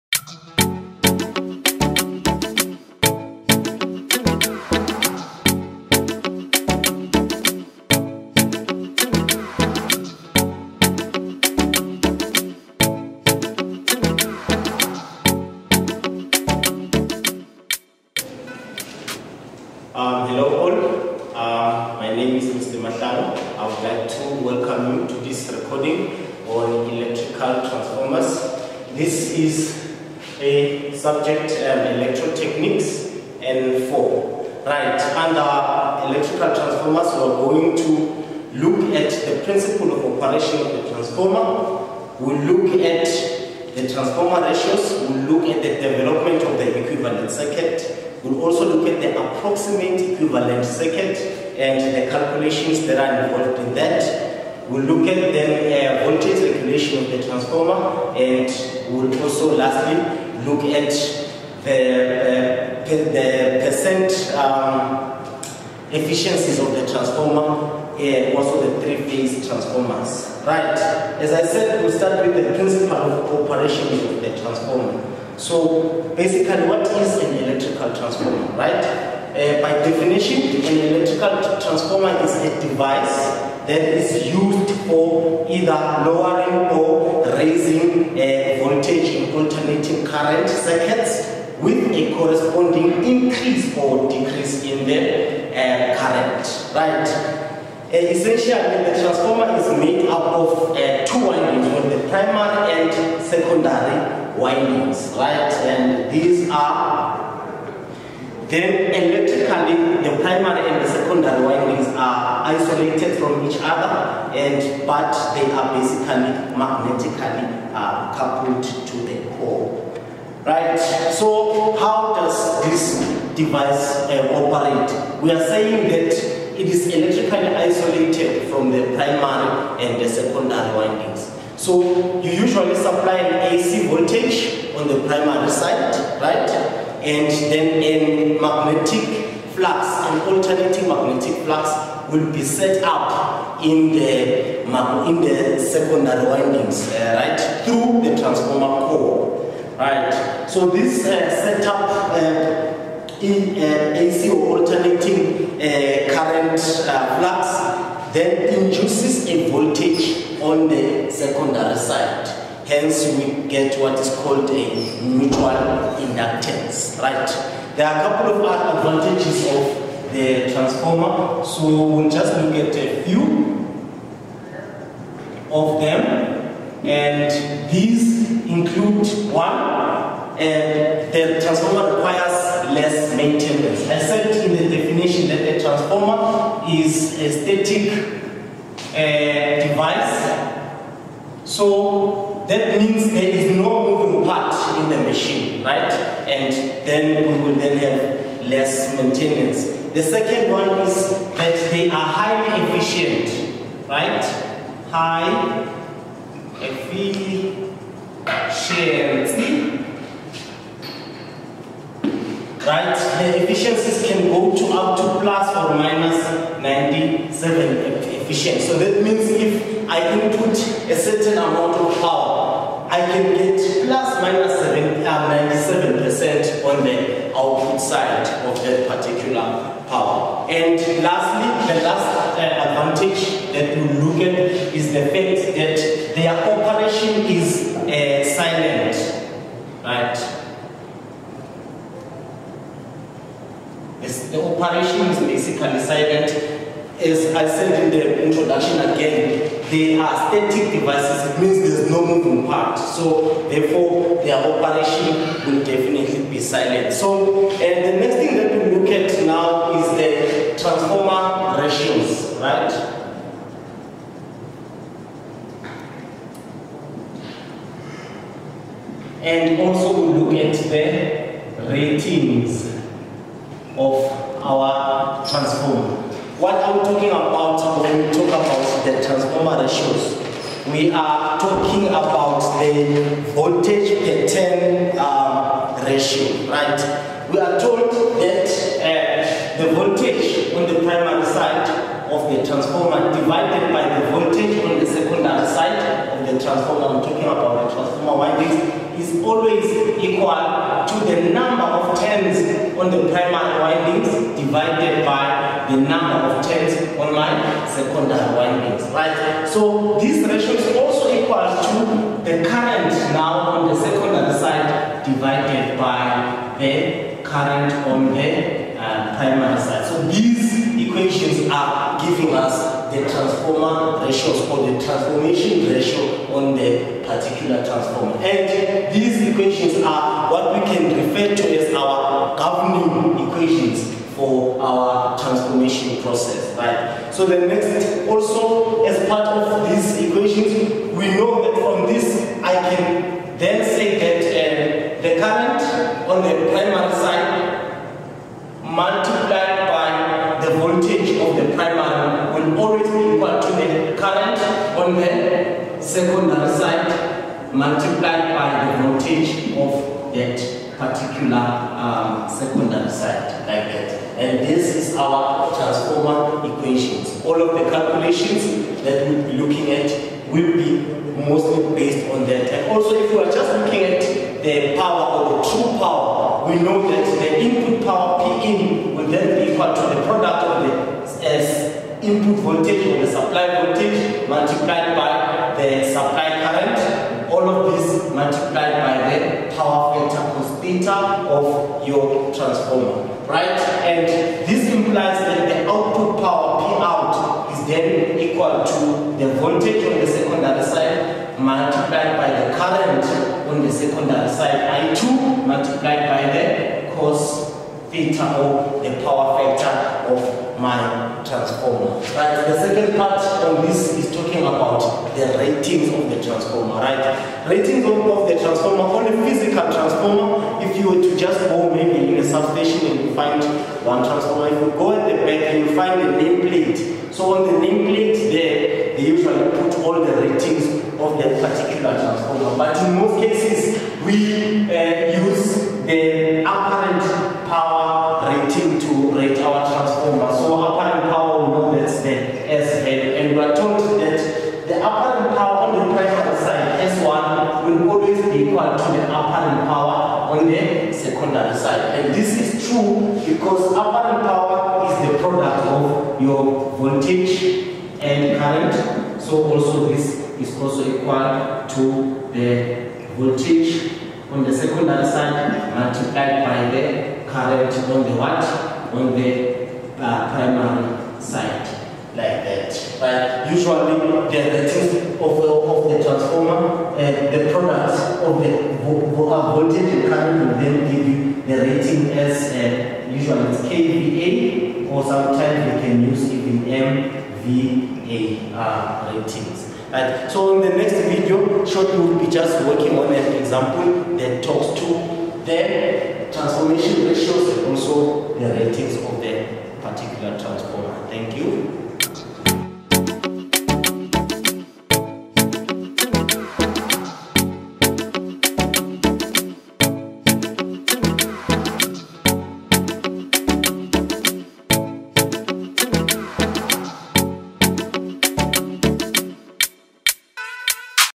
Electrotechnics N4 - N5: Electrical Transformers EP2
Continuing from the last video we look at Electrical Transformers for Electrotechnics N4 students. Join Mr. TD Mahlangu on this exciting lesson.